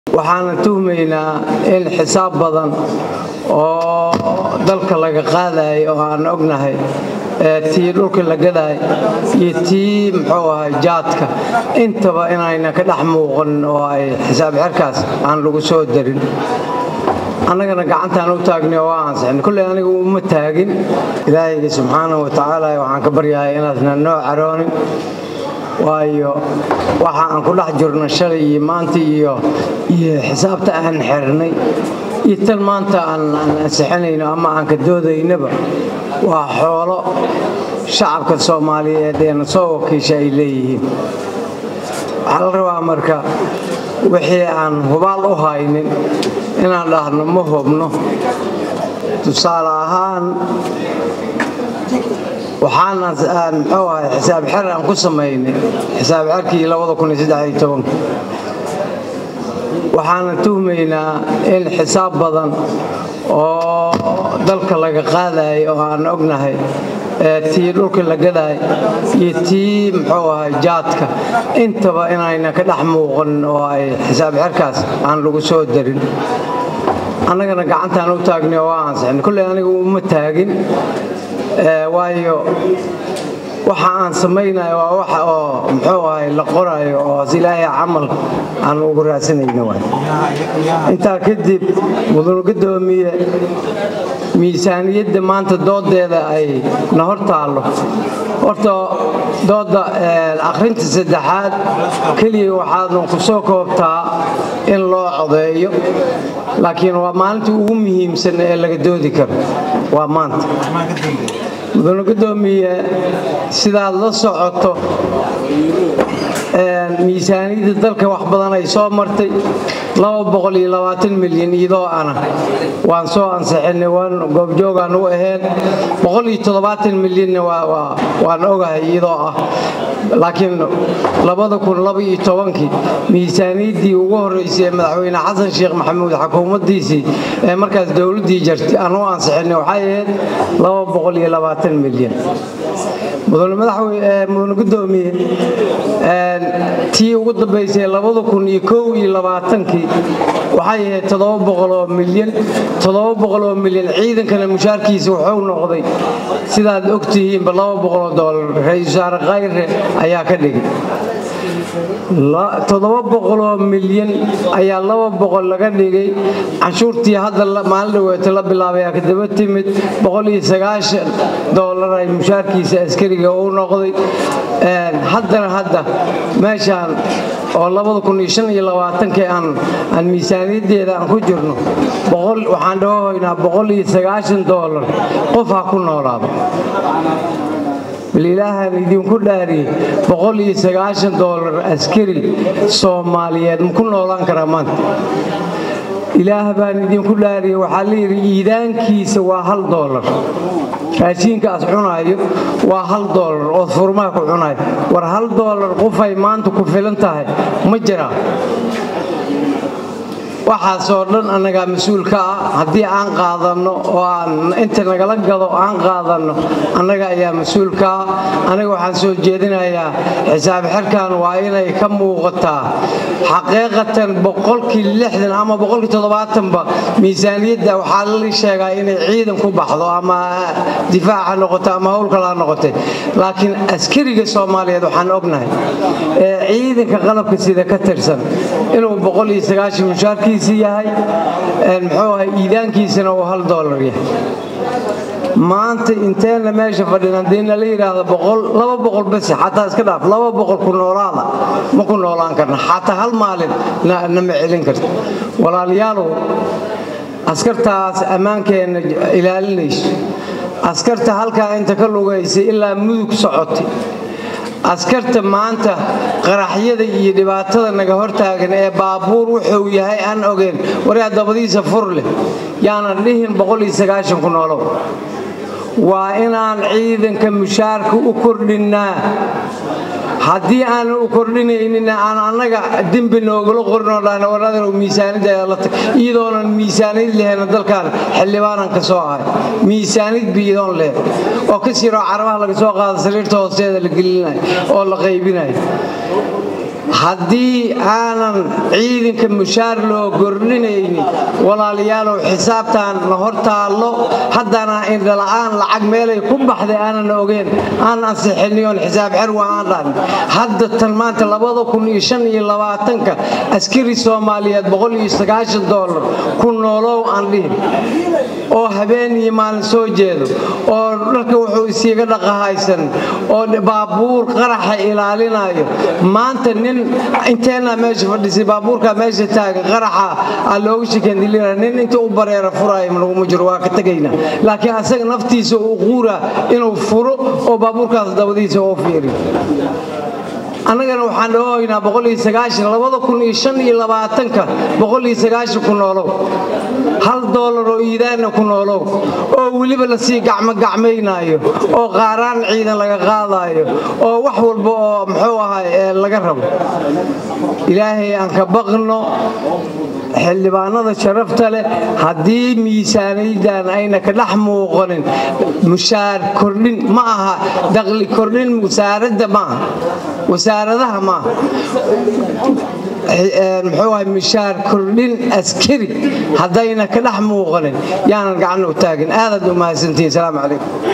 "Waxaan tuhmeynaynaa tii dhulka la gadaayey iyo tan Jaadka aysan ka dhex muuqan xisaabxidhka......"
"Waxaan tuhmeynaynaa tii dhulka la gadaayey iyo tan Jaadka aysan ka dhex muuqan xisaabxidhka, aniguna gacanta uma taagin"
Xil Daahir Amiin Jeesoow, BFS